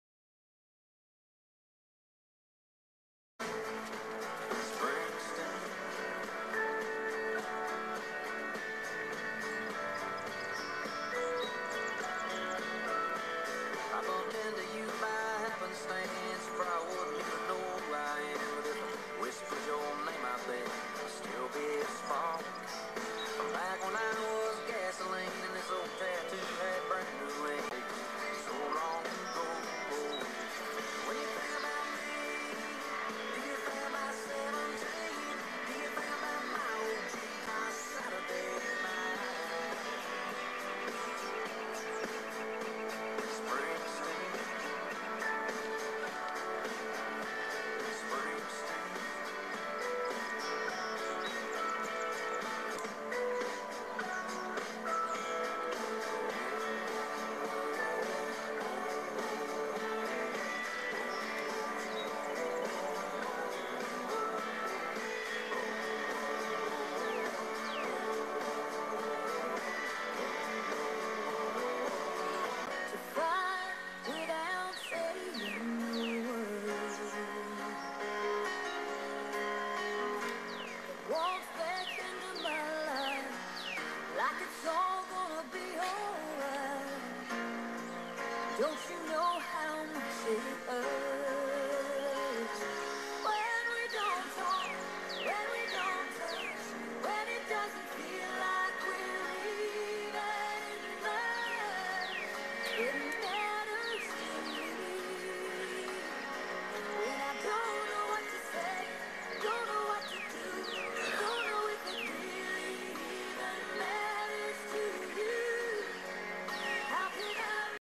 27" x 5/8" Round Bar Bend
I edited this video to cut down the time by removing parts where I just stand there like an idiot. There were two problems with this bend. 1) This was the end of my work out and I was exhausted and 2) I was trying to beat the storm that was coming my way. These two things led to horrible form making this bend a lot harder than it had to be. I was still pleased with my training today.